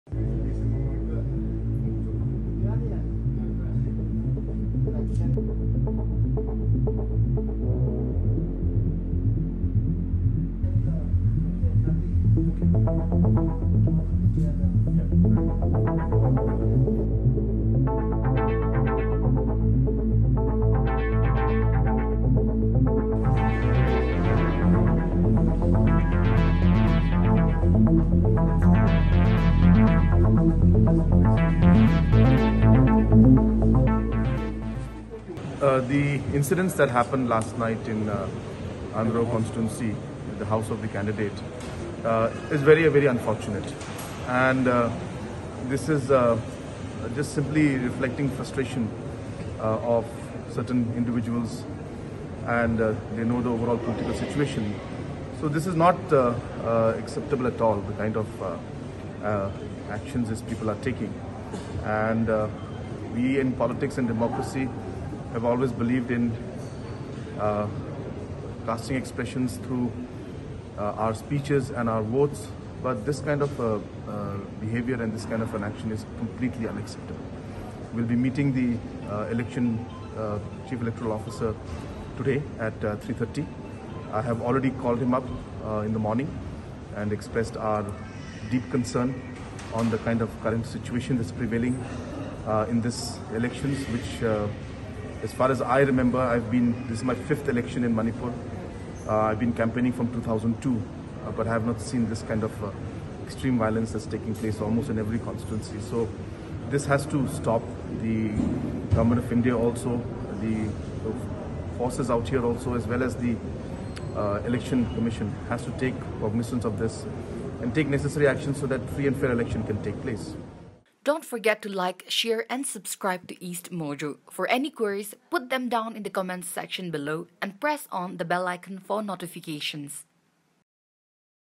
Andro NPP candidate's father shot at during campaign, Meghalaya CM pays visit
NPP candidate from Andro L. Sanjoy Singh’s father was allegedly shot at
in Yairipok Yambem Leikai in Manipur on Friday. Sanjoy's father Shamjai Singh was attending
an election campaign when he was shot on the right hand. He has been admitted to the Shilja Hospital in Imphal. On Saturday, Meghalaya Chief Minister and NPP chief Conrad K Sangma
visited Singh at the hospital.

“This is very unfortunate and is simply reflecting frustration of certain individuals. This is not acceptable at all, the kind of actions these people are taking,” said Sangma.